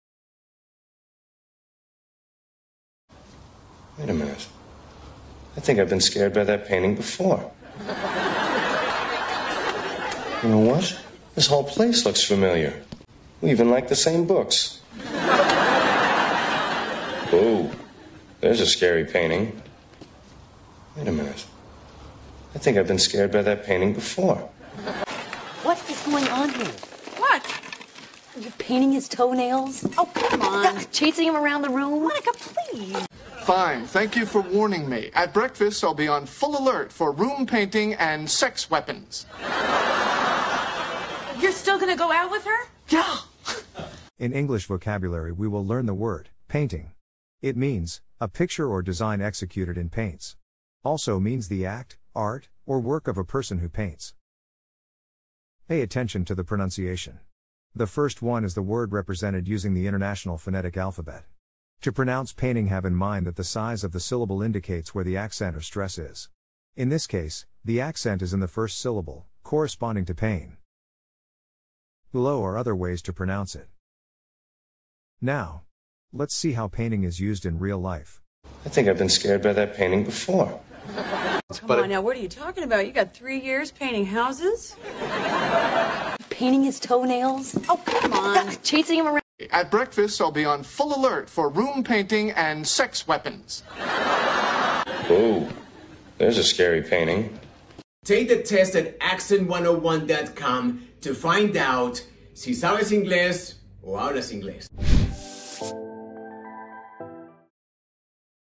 Advanced Anglish vocabulary: 'PAINTING'
En esta Clase de inglés aprenderemos la palabra PAINTING (pintura) con ejemplos de series de televisión

PAINTING significa:

- A picture or design executed in paints.
- The act, art, or work of a person who paints.
- The works of art painted in a particular manner, place, or period: a book on Flemish painting.
- An instance of covering a surface with paint.
- A substance composed of solid coloring matter suspended in a liquid medium and applied as a protective or decorative coating to various surfaces, or to canvas or other materials in producing a work of art.
- An application of this.
- The dried surface pigment: Don't scuff the paint.
- The solid coloring matter alone; pigment.
- Facial cosmetics, especially lipstick, rouge, etc., designed to heighten natural color.
-  Chiefly Western U.S.
- A pied, calico, or spotted horse or pony; pinto.
- To coat, cover, or decorate (something) with paint: to paint a fence.
- To produce (a picture, design, etc.) in paint: to paint a portrait.
- To represent in paint, as in oils, tempera, or watercolor: to paint an actress as the Muse of tragedy.
- To depict as if by painting; describe vividly in words: The ads painted the resort as a winter wonderland.
- To color by or as if by painting: Sunset painted the clouds pink.
- To apply a substance to, as a liquid medicine or a cosmetic: to paint a cut with iodine.
- To coat or cover anything with paint.
- To engage in painting as an art: She has begun to paint in her spare time.
- To put on or use facial cosmetics.
- The art or process of applying paints to a surface such as canvas, to make a picture or other artistic composition
- A composition or picture made in this way
- The act of applying paint to a surface with a brush
- A substance used for decorating or protecting a surface, esp a mixture consisting of a solid pigment suspended in a liquid, that when applied to a surface dries to form a hard coating
- A dry film of paint on a surface
- The solid pigment of a paint before it is suspended in liquid
- Informal
- Face make-up, such as rouge
- Short for greasepaint
- To make (a picture) of (a figure, landscape, etc) with paint applied to a surface such as canvas
- To coat (a surface) with paint, as in decorating
- (tr)
- To apply (liquid) onto (a surface)
- (tr)
- To apply make-up onto (the face, lips, etc)
- (tr)
- To describe vividly in words
- Paint the town red informal
- To celebrate uninhibitedly; go on a spree
- A solution or suspension of one or more medicaments applied to the skin with a brush or large applicator.
- To apply medicine to; swab.

Let's see how 'PAINTING' is used in Advanced English vocabulary:

- a book on Flemish painting. : un libro sobre pintura flamenca.
- Don't scuff the paint. : No raspe la pintura.
- to paint a fence. : pintar una valla.
- to paint a portrait. : pintar un retrato.
- to paint an actress as the Muse of tragedy. : pintar a una actriz como la musa de la tragedia.

Para aprender es importante saber el vocabulario en inglés con promunciación, PAINTING se pronuncia ˈpeɪntɪŋ en el IPA o [ peyn-ting ]